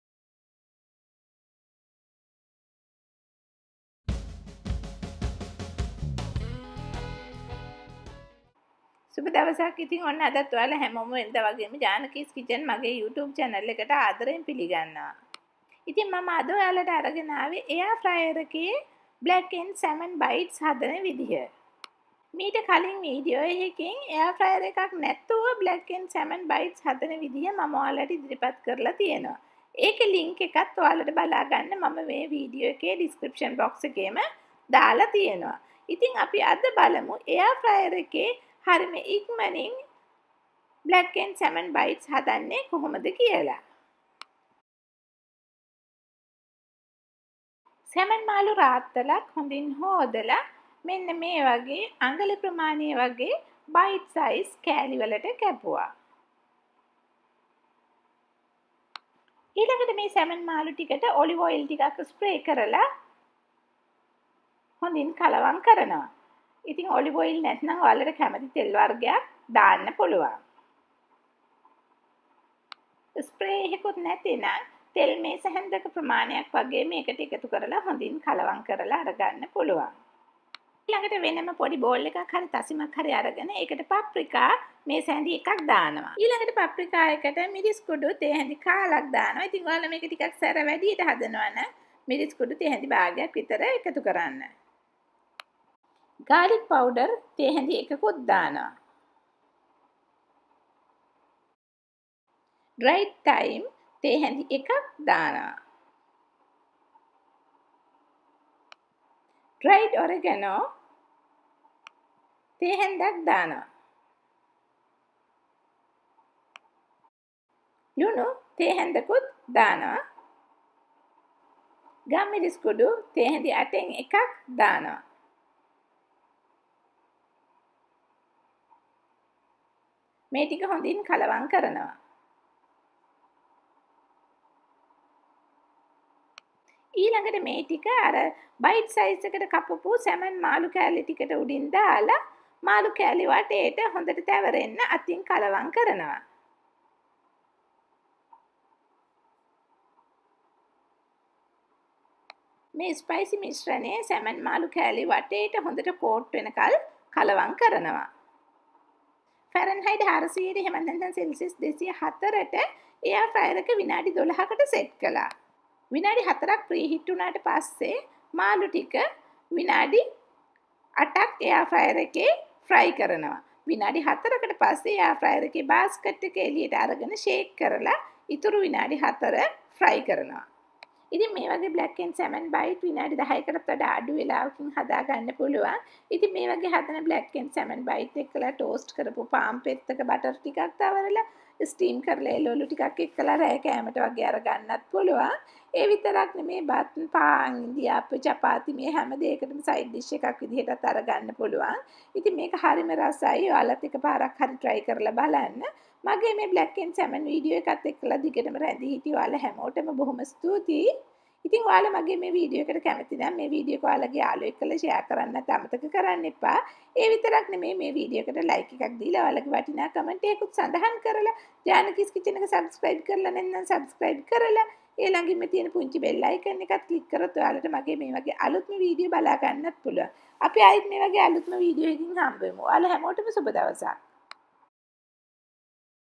සැල්මන් මාළු මෙහෙම හැදුවාමත් මරු :: එයාෆ්‍රයිඩ් බ්ලැක්එන්ඩ් සැල්මන් ⎮AIR FRIED BLACKEND SALMON BITES
These Blackend Salmon Bites pack a very bold punch of flavor and ready in lass than 10 minutes!

Ingredients:
❏  4-6 Ounce Salmon Fish - Diced in 1 Inch
❏  Oil Spray or 1 Tbsp Oil
❏  1 Tbsp Paprica